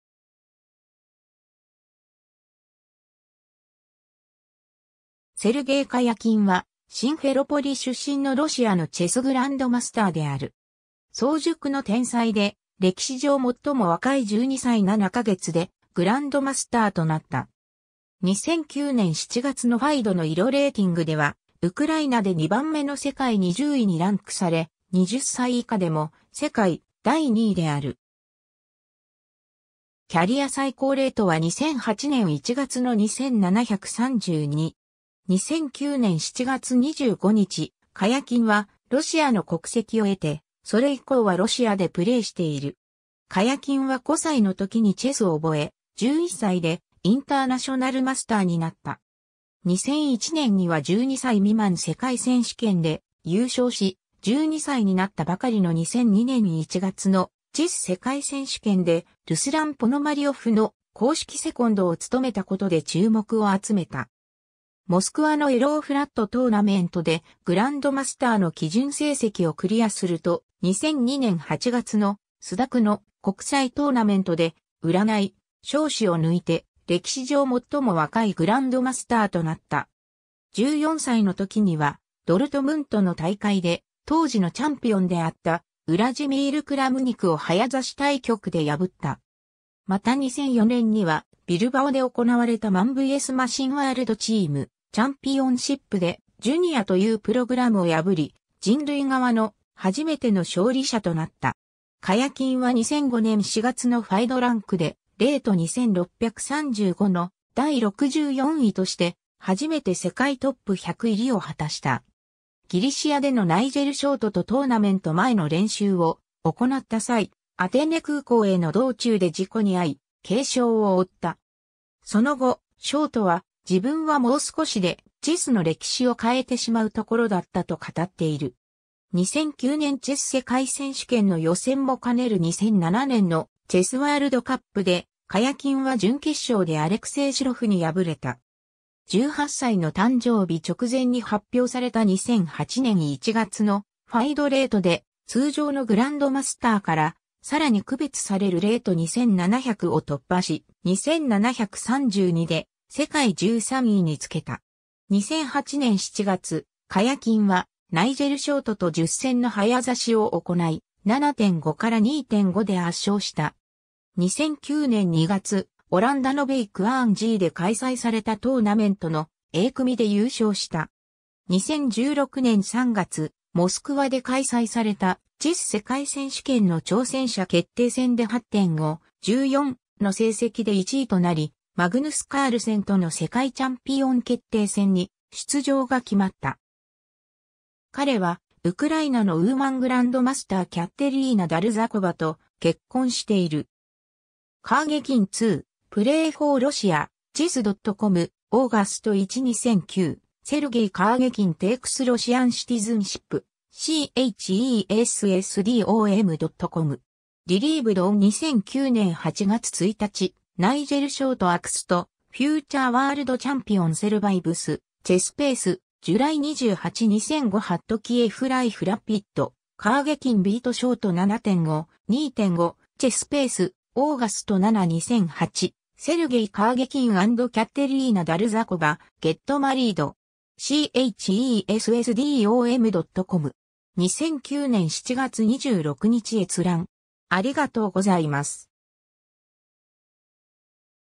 セルゲイ・カヤキン
セルゲイ・カヤキン（Sergey Karjakin、ロシア語:Сергей Карякин、ウクライナ語:Сергій Карякін、1990年1月12日‐）は、シンフェロポリ出身のロシア（以前はウクライナ）のチェスグランドマスターである。
早熟の天才で、歴史上最も若い12歳7カ月でグランドマスターとなった。
2009年7月のFIDEのイロレーティングでは、ウクライナで2番目の世界20位にランクされ、20歳以下でも世界第2位である。
キャリア最高レートは2008年1月の2732。
 2009年7月25日、カヤキンはロシアの国籍を得て、それ以降はロシアでプレーしている。
 カヤキンは5歳の時にチェスを覚え、11歳でインターナショナルマスターになった。
2001年には12歳未満世界選手権で優勝し、12歳になったばかりの2002年1月のチェス世界選手権でルスラン・ポノマリオフの公式セコンドを務めたことで注目を集めた。
モスクワのAeroflotトーナメントでグランドマスターの基準成績（ノーム）をクリアすると、2002年8月のスダクの国際トーナメントで、卜祥志を抜いて歴史上最も若いグランドマスターとなった。
 14歳の時には、ドルトムントの大会で当時のチャンピオンであったウラジミール・クラムニクを早指し対局（持ち時間10分＋1手5秒）で破った。
また2004年には、ビルバオで行われたMan vs Machine World Team Championshipで「ジュニア」というプログラムを破り、人類側の初めての勝利者となった。
 カヤキンは2005年4月のFIDEランクで、レート2635の第64位として、初めて世界トップ100入りを果たした。
ギリシアでのナイジェル・ショートとトーナメント前の練習を行った際、アテネ空港への道中で事故にあい、軽傷を負った。
その後、ショートは「自分はもう少しでチェスの歴史を変えてしまうところだった」と語っている。
 2009年チェス世界選手権の予選も兼ねる2007年のチェスワールドカップで、カヤキンは準決勝でアレクセイ・シロフに敗れた。
18歳の誕生日直前に発表 された2008年1月のFIDEレートで、通常のグランドマスターからさらに区別されるレート2700を突破し、2732で世界13位につけた。
 2008年7月、カヤキンはナイジェル・ショートと10戦の早指しを行い、7.5-2.5で圧勝した。
2009年2月、オランダのWijk aan Zeeで開催されたトーナメントのA組で優勝した。
 2016年3月、モスクワで開催されたチェス世界選手権の挑戦者決定戦（8名総当りダブルラウンド）で8.5/14の成績で1位となり、マグヌス・カールセンとの世界チャンピオン決定戦に出場が決まった。
 彼は、ウクライナのウーマングランドマスターKateryna Dolzhikovaと結婚している。
 ^ Karjakin to Play for Russia, Chess.com, August 1 2009 ^ Sergey Karjakin takes Russian citizenship, Chessdom.com. Retrieved on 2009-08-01. ^ Nigel Short axed, future world champion survives, Chessbase, July 28 2005 ^ Kiev Life Rapid: Karjakin beat Short 7.5:2.5, Chessbase, August 7 2008 ^ “Sergey Karjakin and Kateryna Dolzhikova get married”. Chessdom.com. 2009年7月26日閲覧。